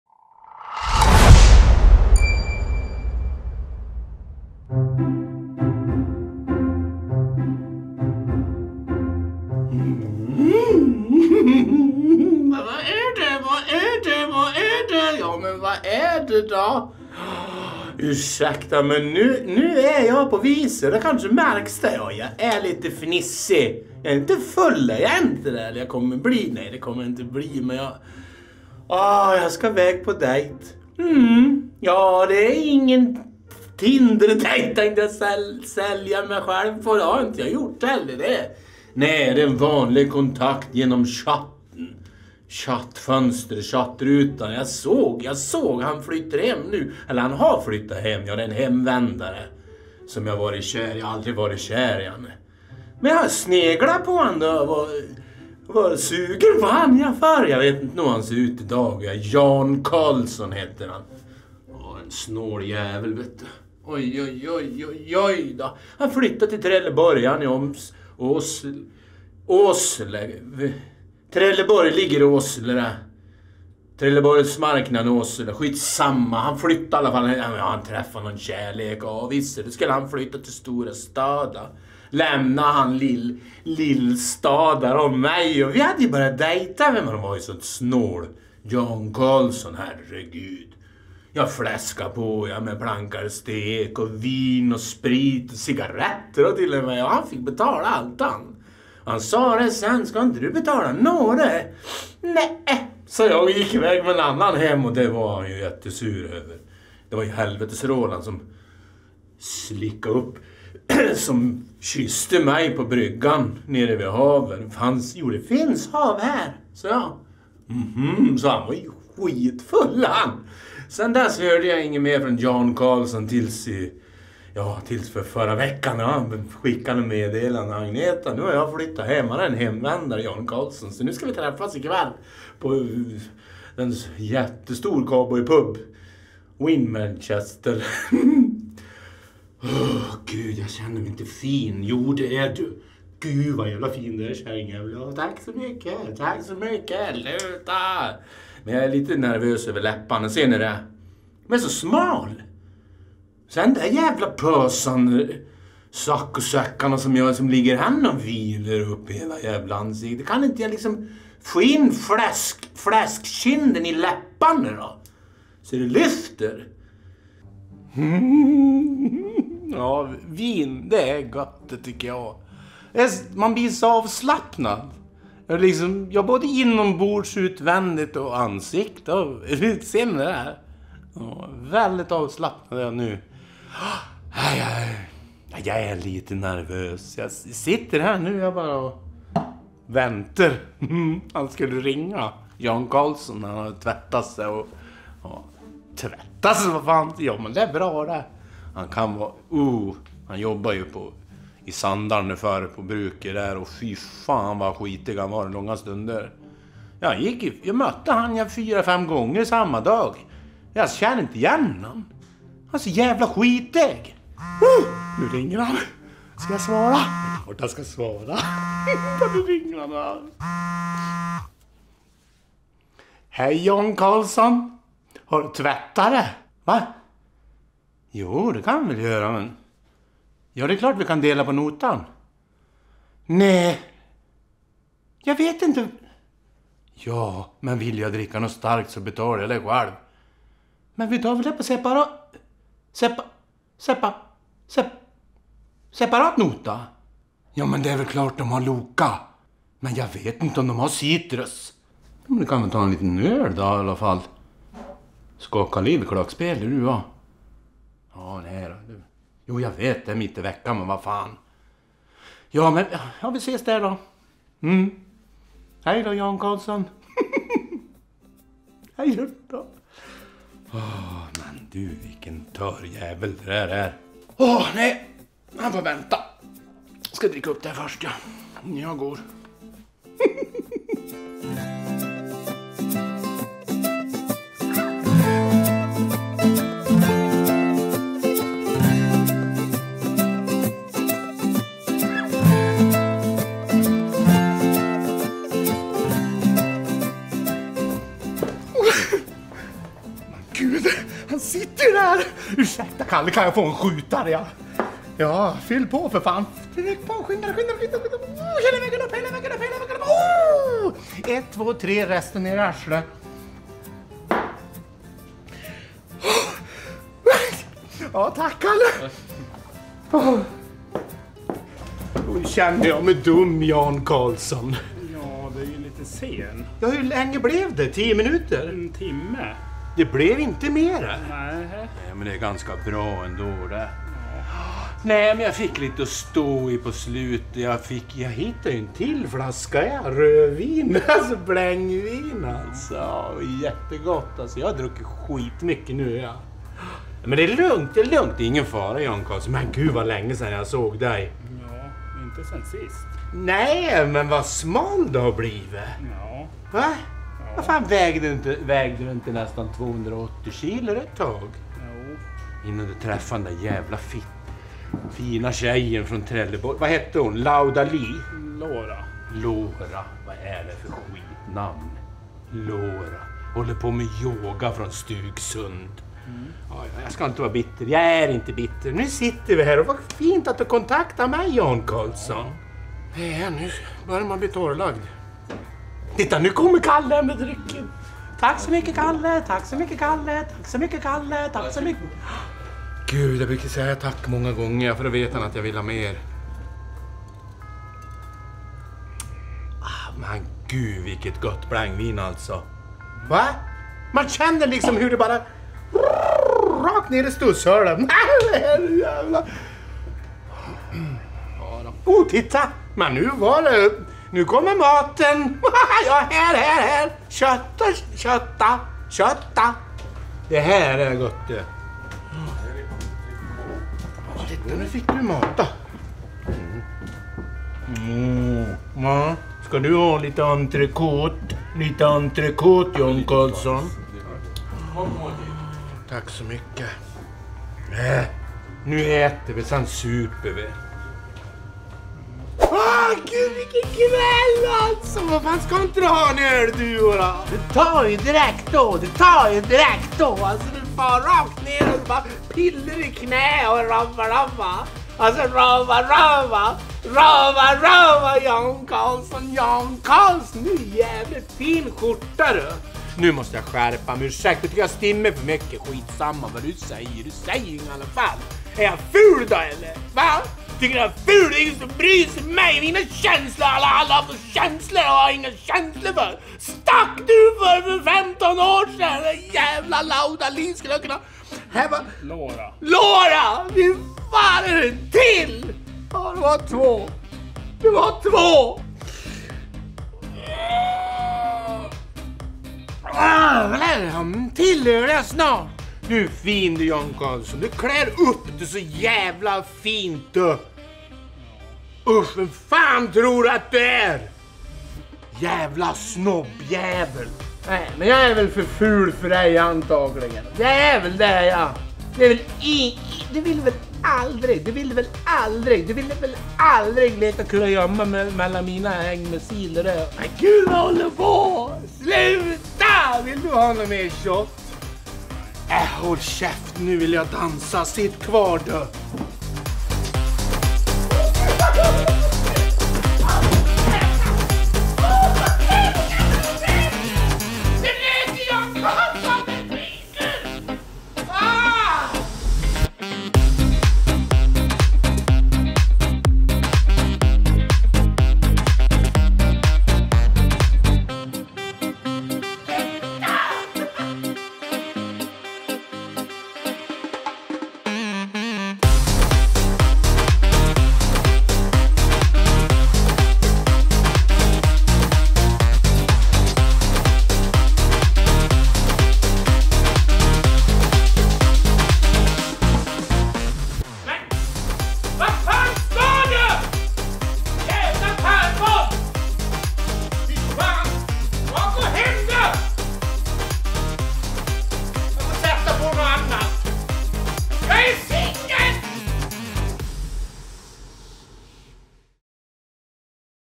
DATE MED JAN KARLSSON
Agneta går på dejt med Jan Karlsson.